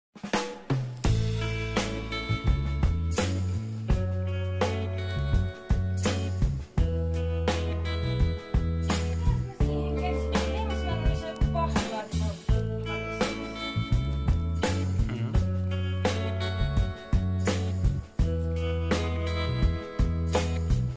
Hillan koiranpennut 14.6.2019 osa 1
Hillan pennut alkaa liikkumaan jo niin paljon että tarvittiin isompi alue niille